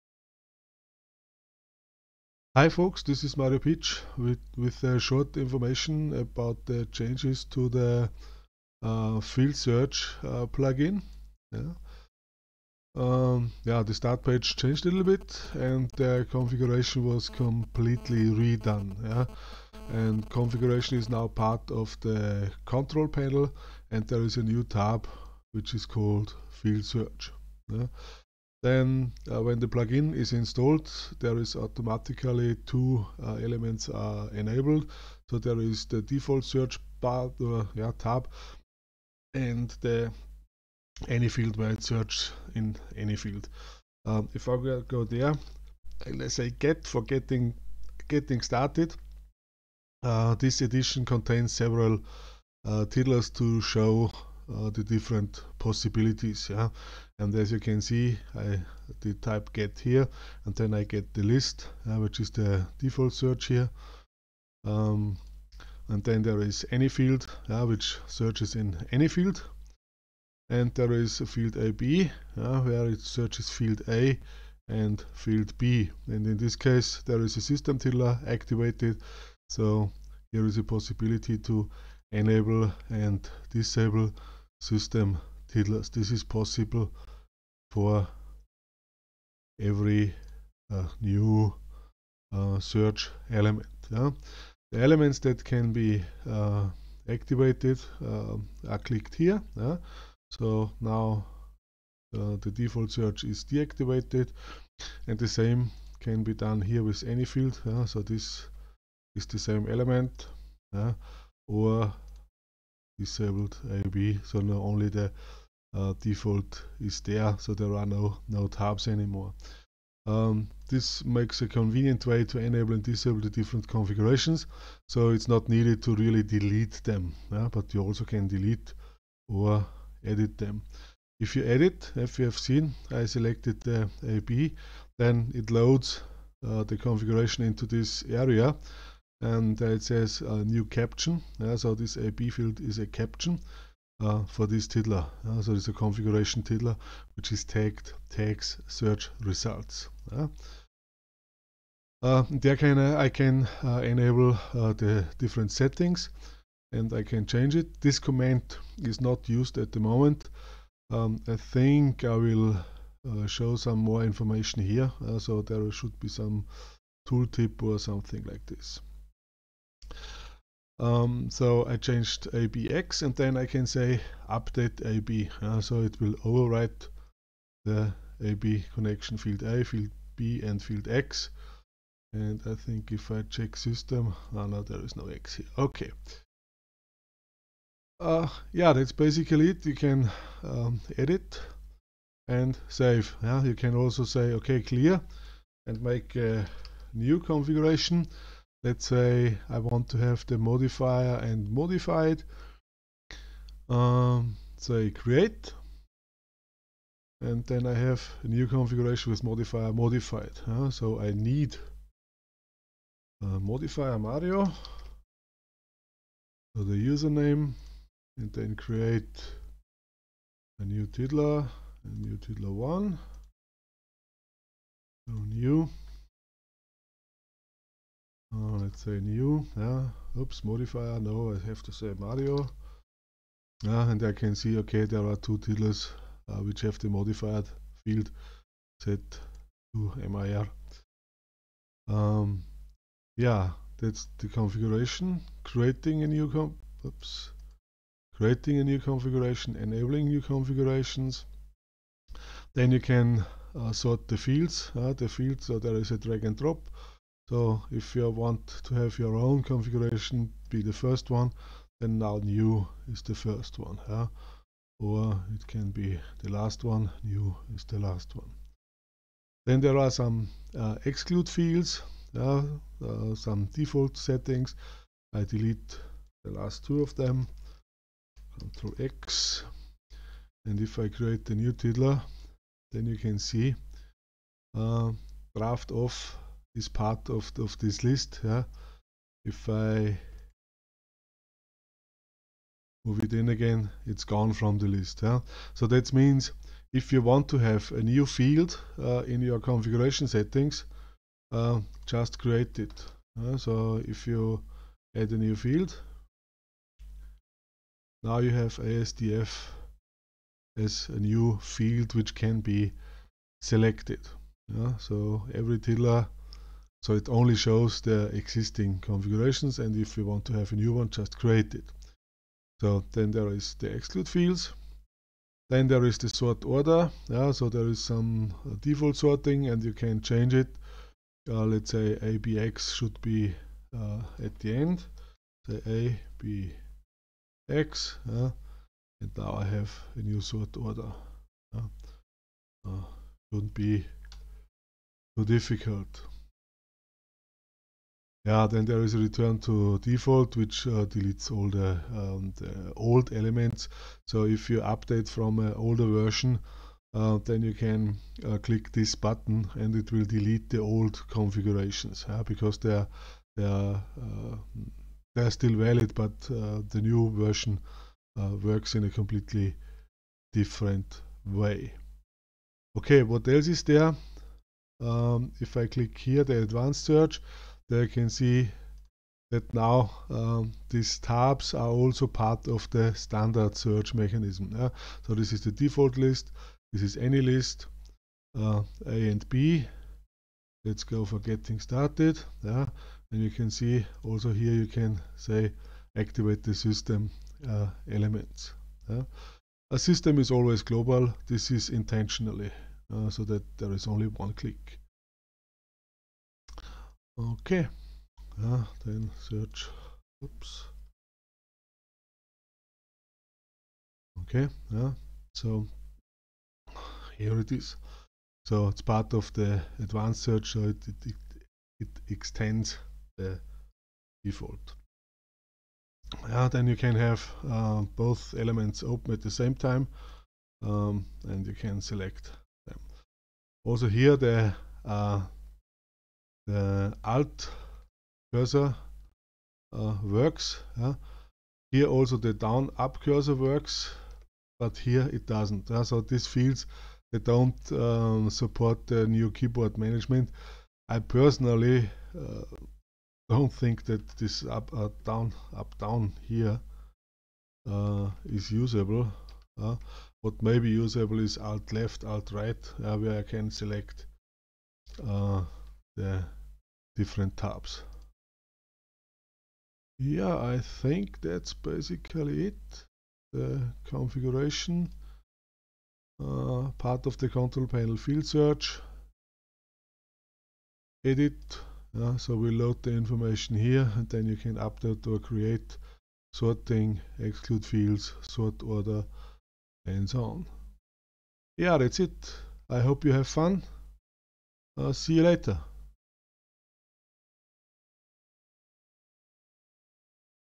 Field Search V0.4.0
Tiddlywiki Field Search Plugin Version 0.4.0 was completely rewritten.